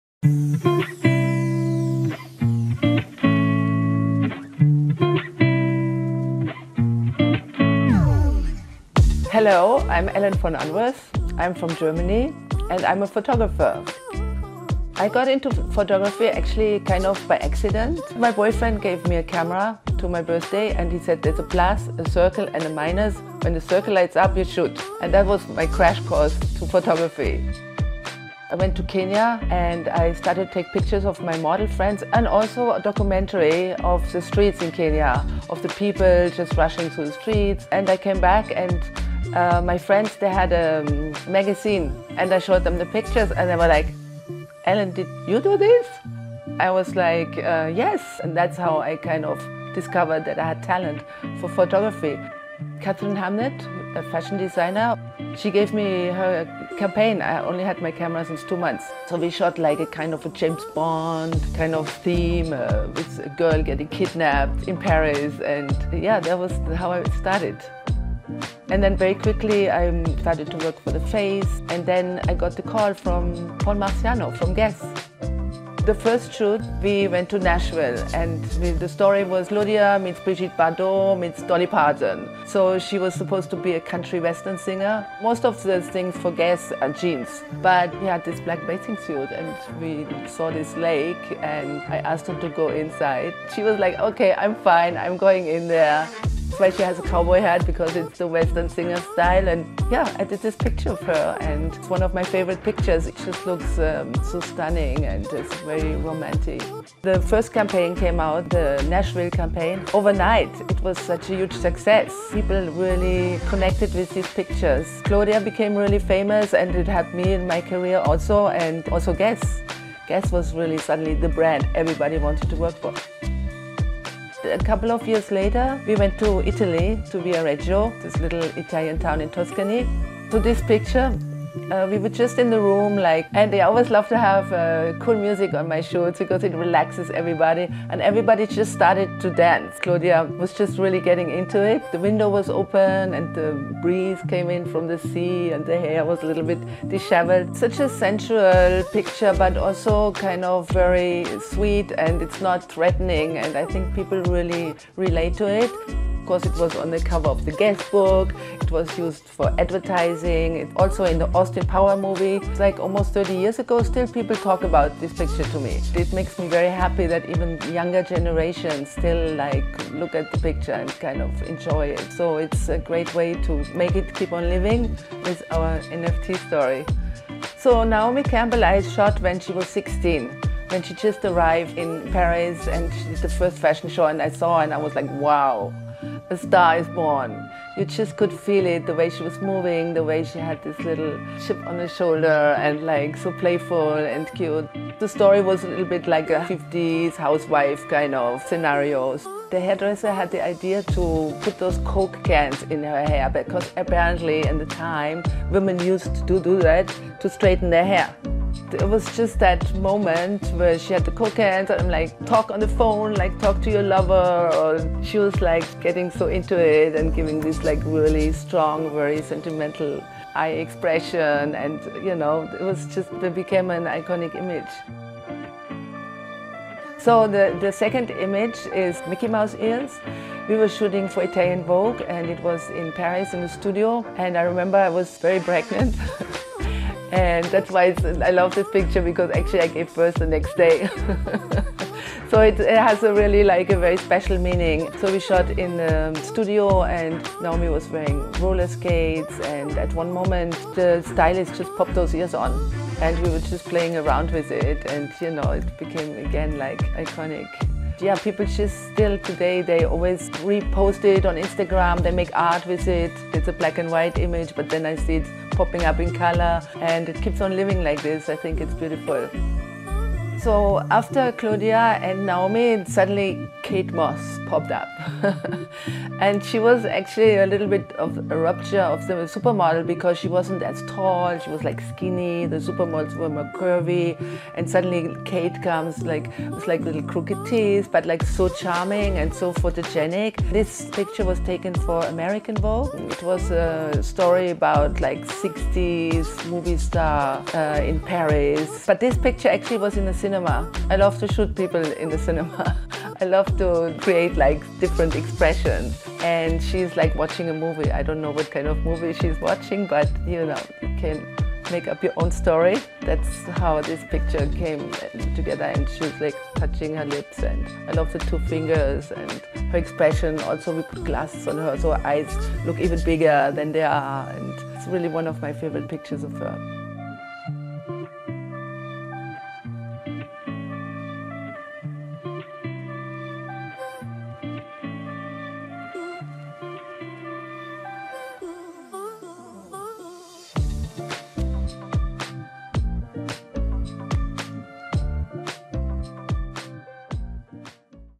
NFT photography: Ellen von Unwerth NFT 'Supermodel Series': Kate Moss and Claudia Schiffer
The 'Supermodel Series' is NOW LIVE 10.21.21 for the next 72 hours exclusively

Known as one of Hollywood’s most elite photographers, Ellen von Unwerth has been capturing stars in delightfully seductive poses for more than forty years. Spending almost ten years as a model in Paris, she experienced life in the forefront of the camera’s lens before partnering with Claudia Schiffer for the 1989 Guess campaign that launched her photography career, one of the NFTs featured in this collection. 📸

Let her take you through the timeless stories behind the scenes with Claudia Schiffer and Kate Moss and their impact on the fashion industry today as she launches her first NFT collection with us: the 'Supermodel Series'. ⭐️